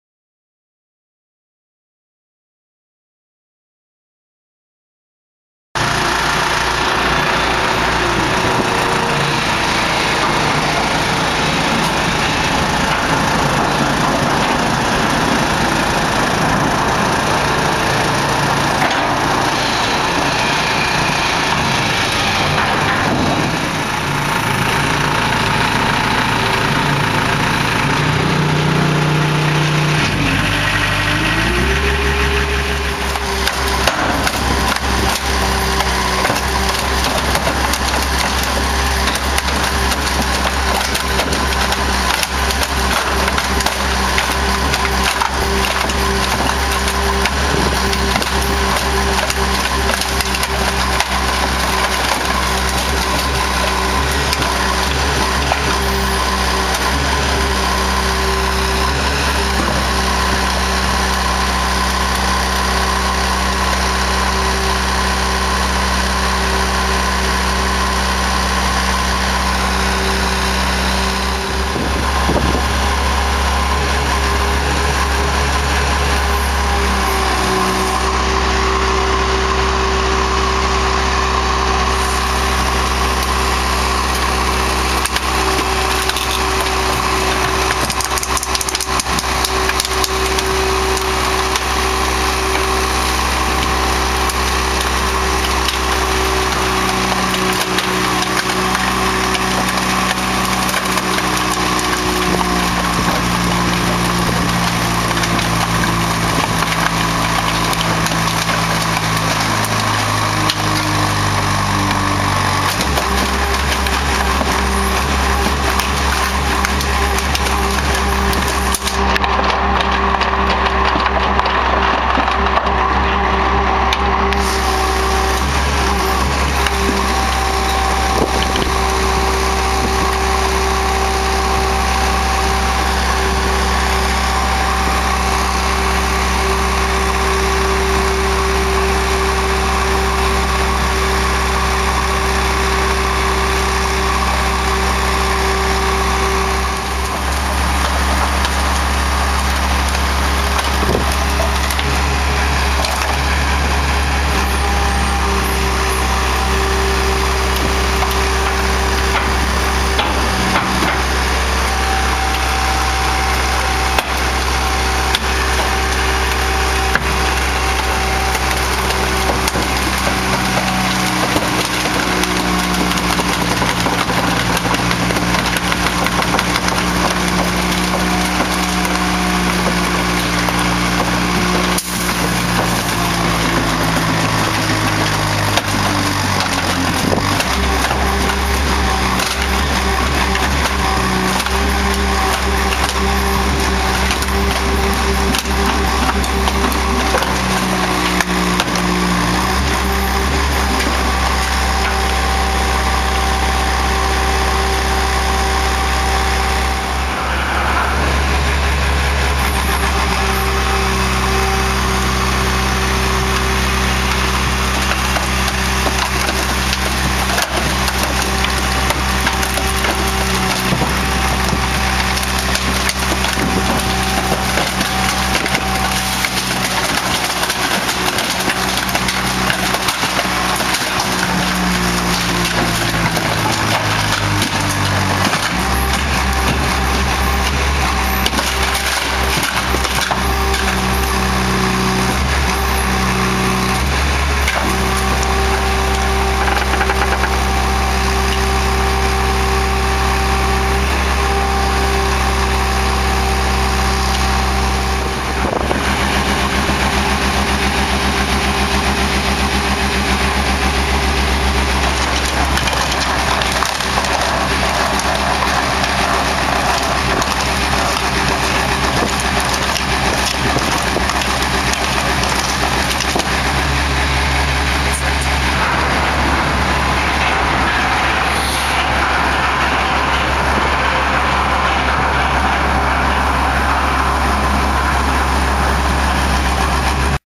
Kelman Engineering Compactor
Stand alone Hydraulic powered scrap car crusher - WITH LOADER
Power pack attached
Simple 2 hand forward/back up/down system
Scrap to CASH more efficiently with less travel costs!!